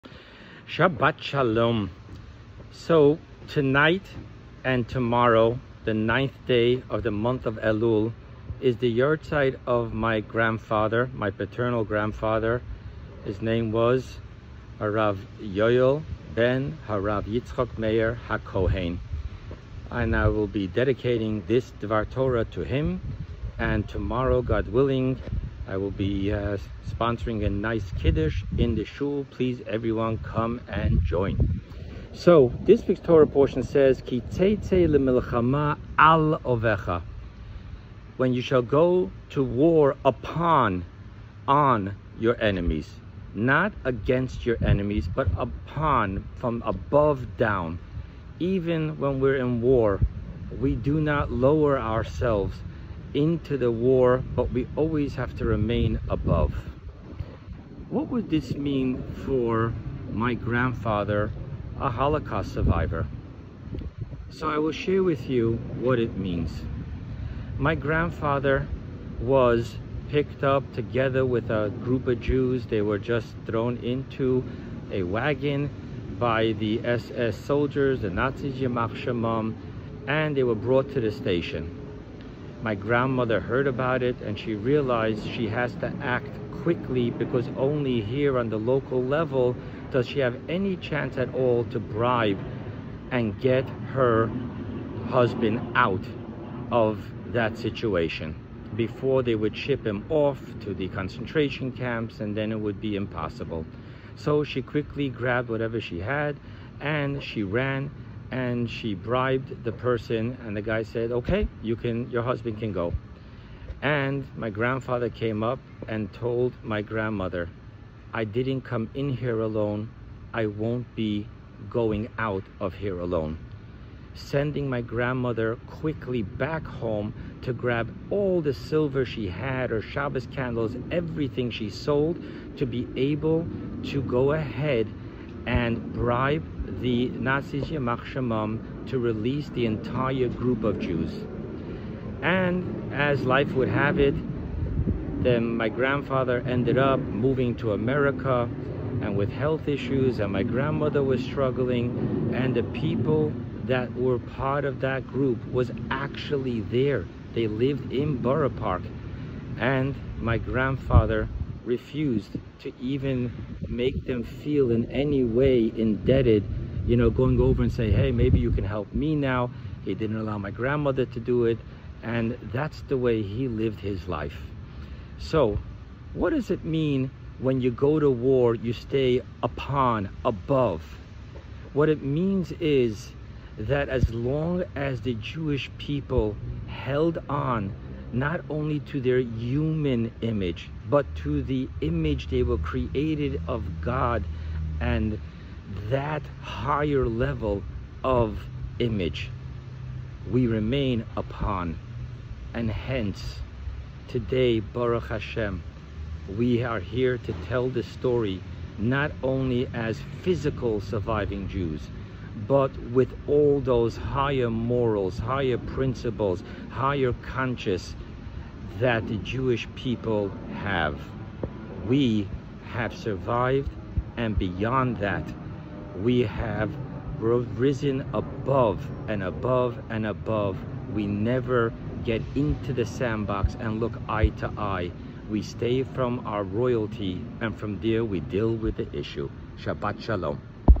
Ki Teitzei: “My Grandfather, and G-s’s Expectations of a Jew When at War”
The Torah commands us, that when we need to go to war, we are not to lower ourselves into an eye-to-eye level with the enemy. But rather, to remain above, in our higher consciousness.

How were my grandfather and all the Jews in the Shoah meant to do this?! Story time!